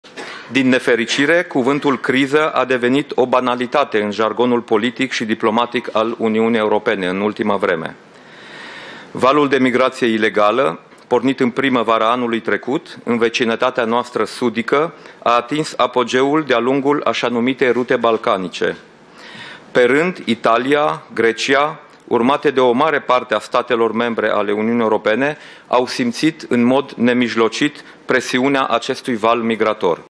Deschiderea oficială a Reuniunii Anuale a Diplomației Române (RADR 2016)
Premierul Dacian Cioloș: Din nefericire, cuv&#226;ntul „criză” a devenit o banalitate &#238;n jargonul politic și diplomatic al Uniunii Europene.
Titlul evenimentului: Deschiderea oficială a Reuniunii Anuale a Diplomației Rom&#226;ne (RADR 2016)